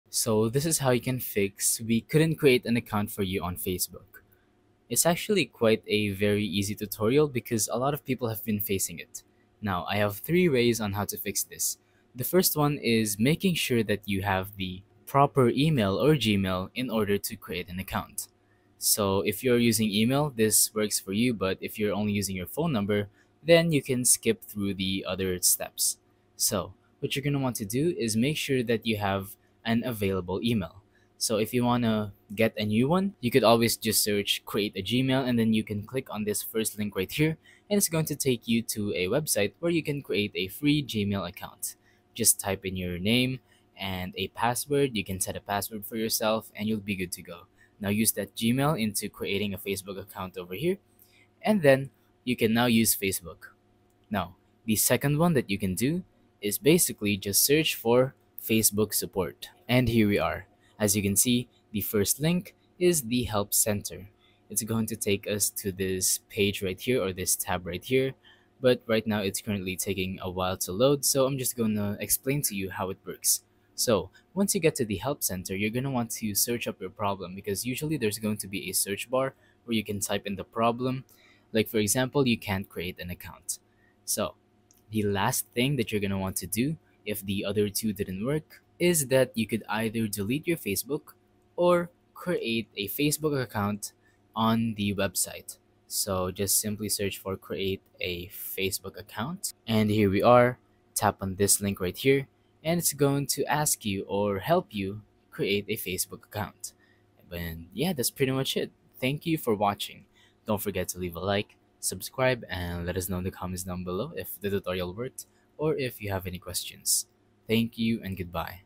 How To Fix We Couldn't Create an Account for You on Facebook
Learn how to fix we couldn't create an account for you on Facebook with this simple guide! We’ll show you how to troubleshoot the Facebook we couldn't create your account problem and solve issues like Facebook something went wrong. Get your account up and running with these quick fixes for Facebook we can't create your account errors!

⭐️ Social Tech Insider is your go-to for tech tutorials, insights, and tips inspired by reliable channels!

Social Tech Insider